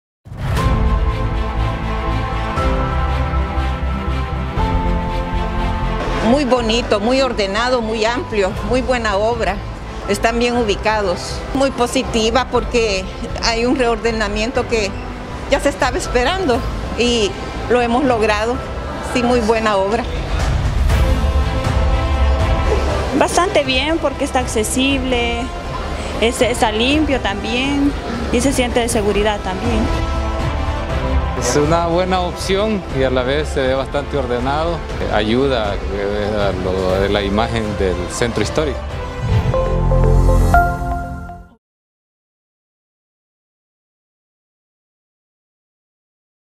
Más de 3,000 capitalinos visitan el mercado Hula Hula
Más de 3,000 capitalinos visitan el mercado Hula Hula en el primer fin de semana de operaciones oficiales.